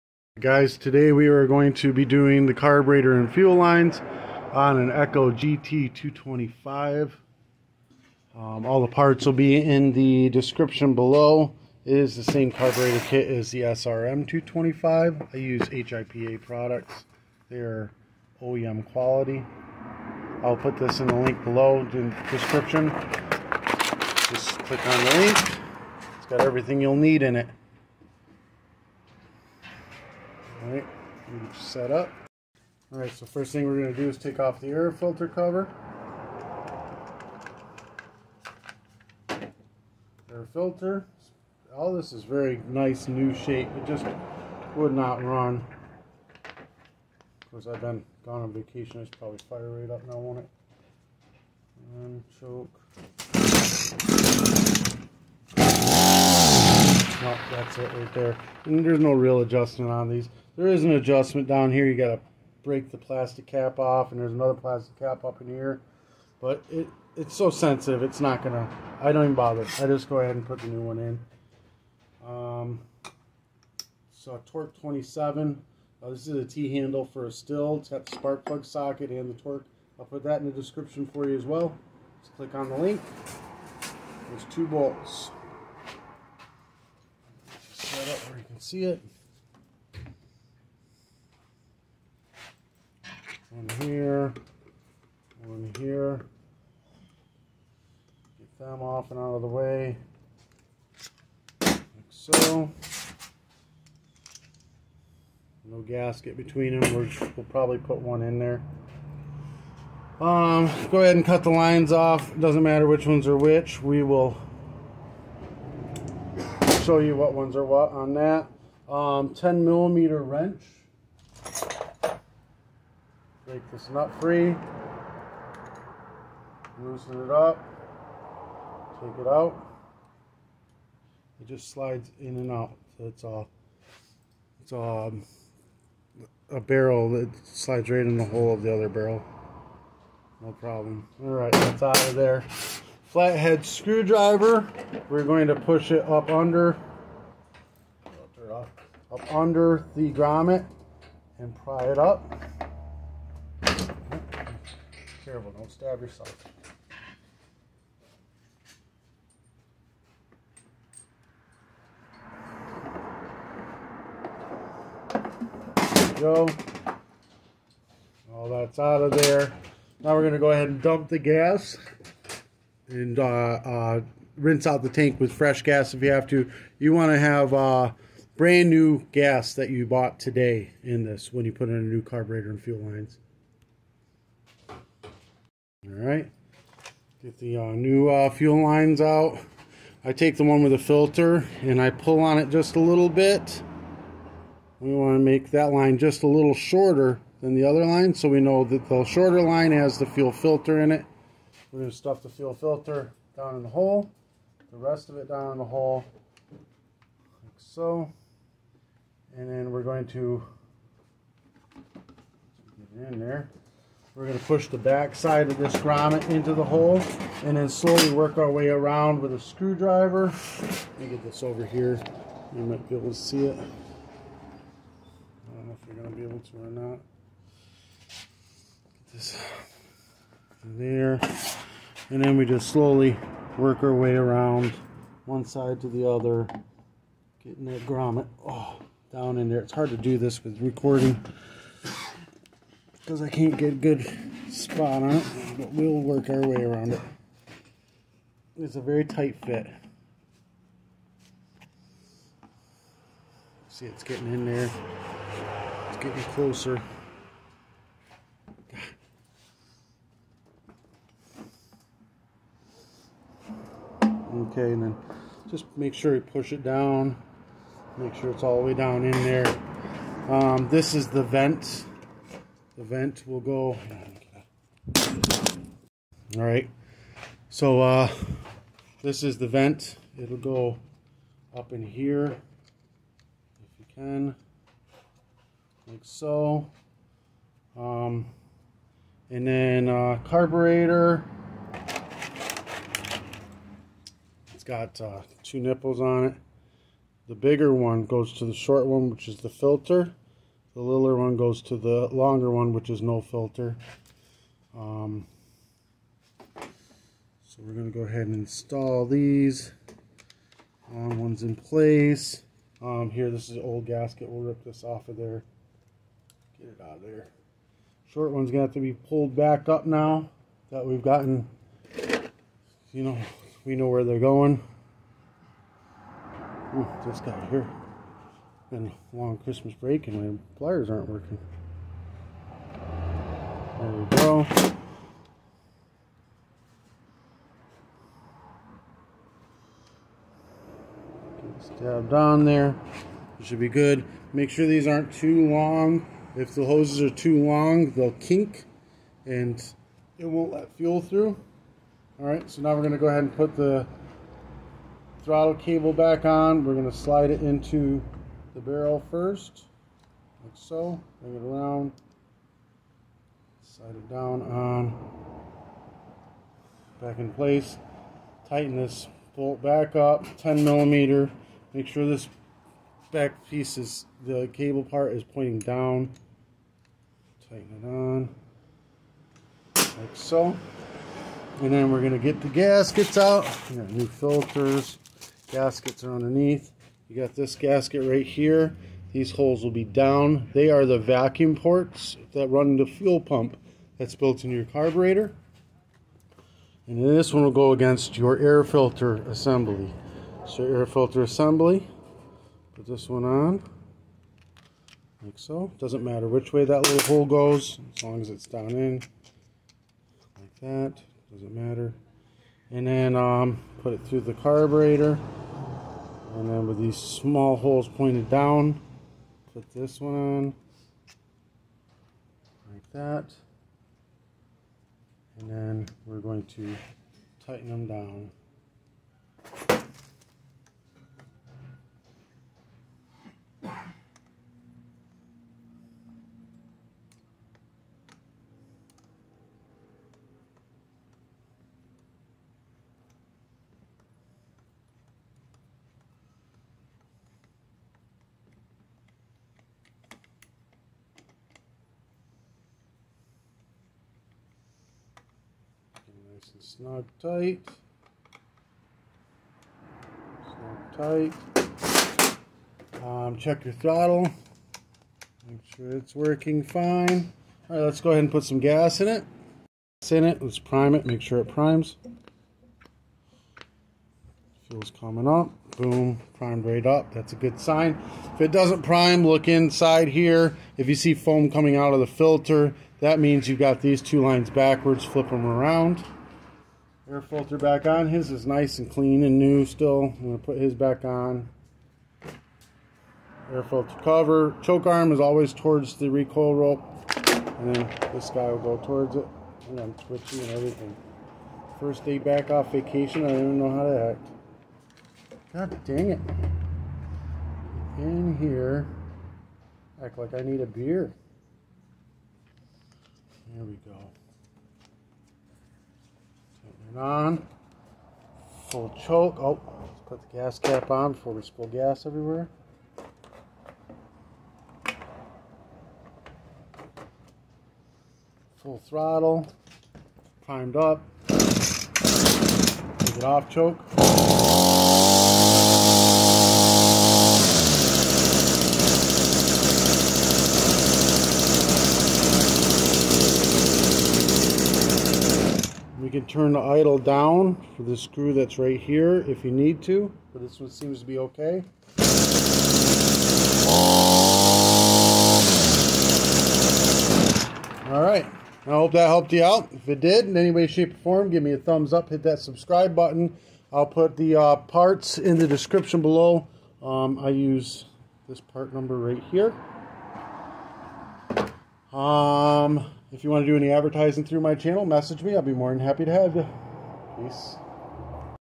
echo gt-225 trimmer carburetor fuel line repair replace kit install HIPA
HIPA carb kit